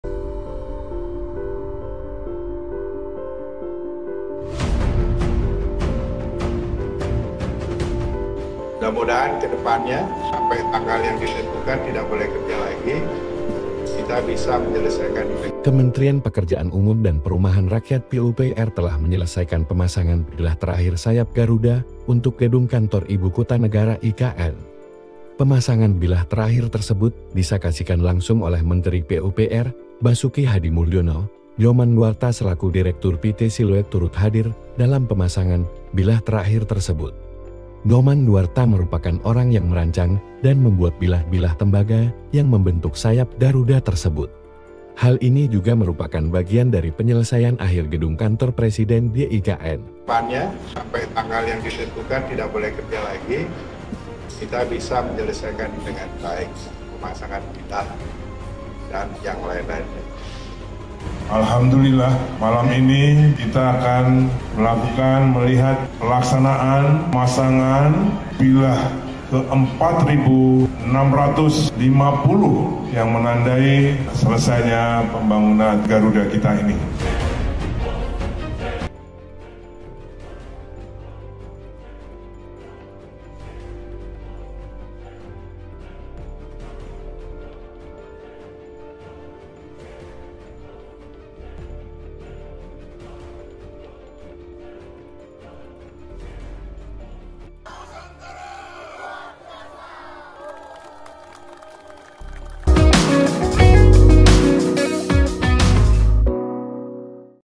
Detik-detik Pemasangan Bilah Terakhir Sayap Garuda di IKN
Kementrian Pekerjaan Umum dan Perumahan Rakyat (PUPR) telah menyelesaikan pemasangan bilah terakhir sayap Garuda untuk Gedung  Kantor Ibu Kota Negara (IKN).

Pemasangan bilah terakhir tersebut disakasikan langsung oleh Menteri PUPR, Basuki Hadimuljono. Nyoman Nuarta selaku Direktur PT Siluet turut hadir dalam pemasangan bilah terakhir tersebut.

Nyoman Nuarta merupakan orang yang merancang dan membuat bilah-bilah tembaga yang membentuk sayap Garuda tersebut.

Hal ini juga merupakan bagian dari penyelesaian akhir gedung Kantor Presiden di IKN.
Video terkini dan terbaru..